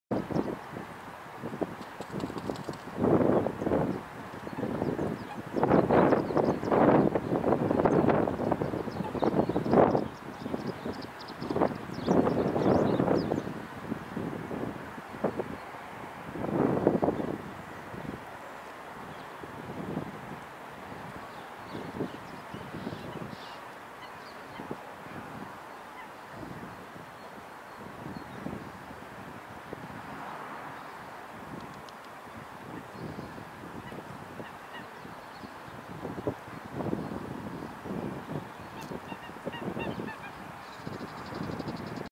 Pied Wagtails in the rain
Two Pied Wagtails hunt for insects beside the Round Pond in the rain.